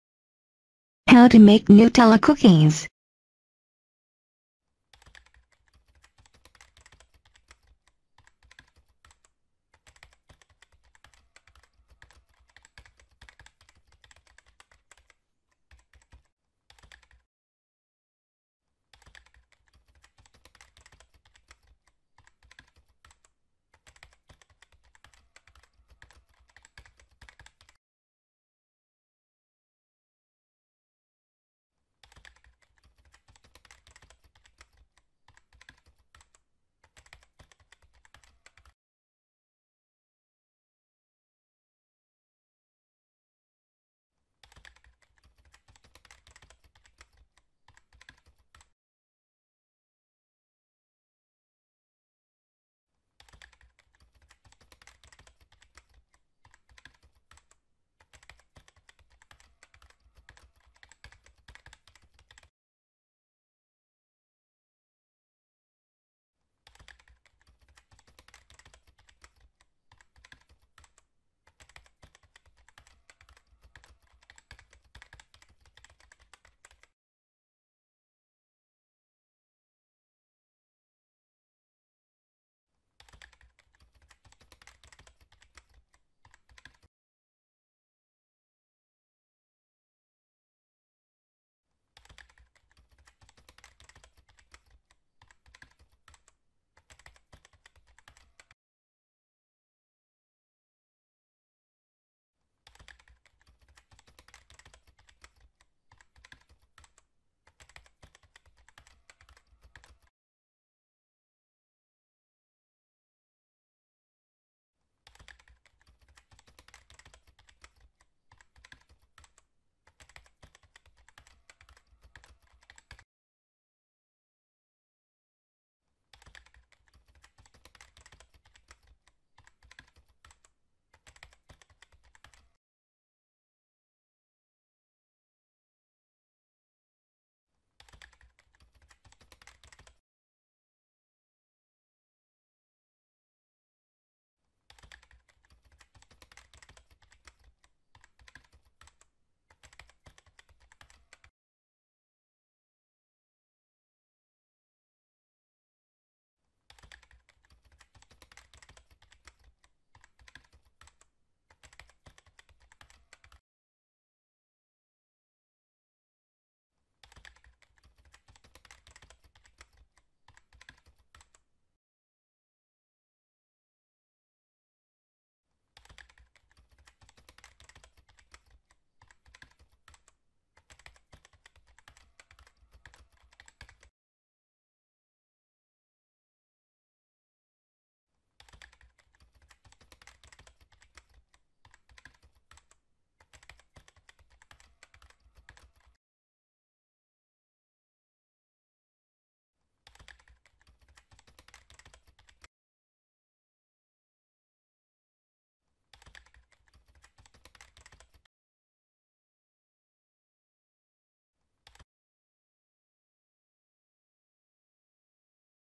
How to Make Nutella Cookies Easy | How to Make Nutella Cookies at Home
In this video you learn how to make Nutella Cookies at home after watching this video you are able to make Nutella Cookies at home. so please watching Nutella Cookies recipies and be able to make Nutella Cookies.